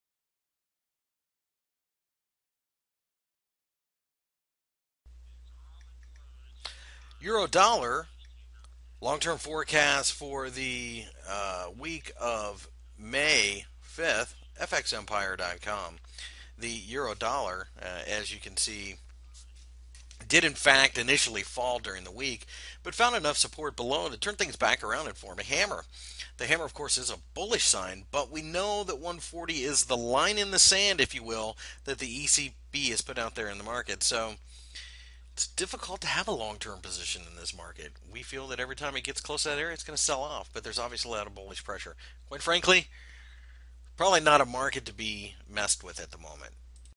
EUR/USD Forecast for the week of May 5, 2014, Technical Analysis
- May 5, 2014 currency weekly technical analysis for the EUR/USD pair.Find more information about Forex News Forex Technical Analysis Forex Fundamental Analysis and Forex Brokers on FX Empire